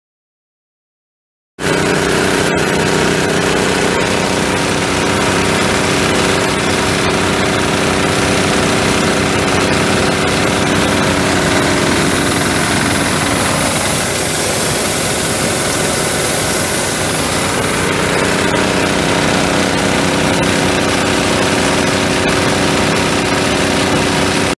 Ingersoll Rand PSIG air compressor for sale | no-reserve Internet auction October 25, 2017
Ingersoll Rand PSIG air compressor
Honda GX160 single cylinder gas engine
11.8 cfm
90 max psi
Hose reel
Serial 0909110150
It's located in Plattsburg, MO, and we're selling it to the highest bidder regardless of price.